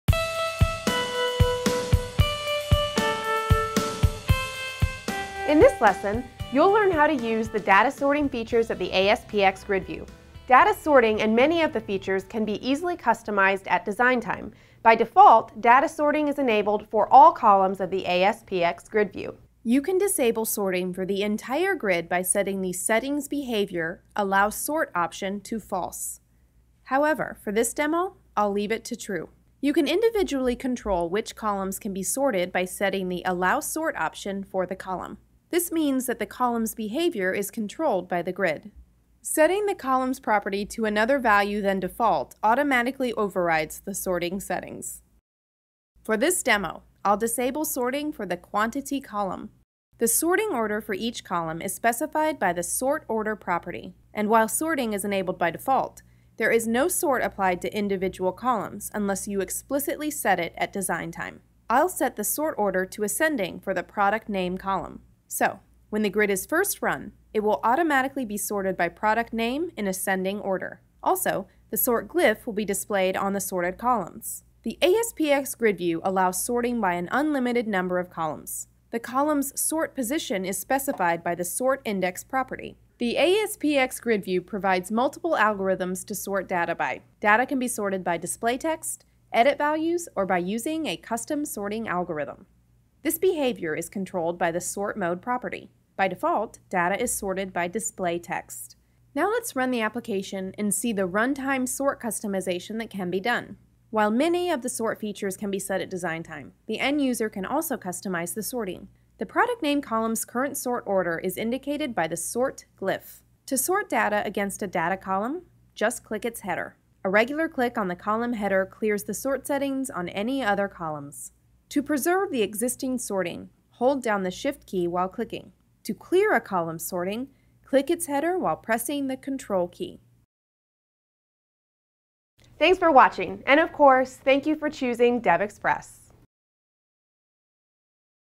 ASP.NET Grid - Data Sorting Basics
This video covers our 2008 release.  Learn how to use data sorting features of the ASPxGridView.  For more information on our subscriptions, please visit...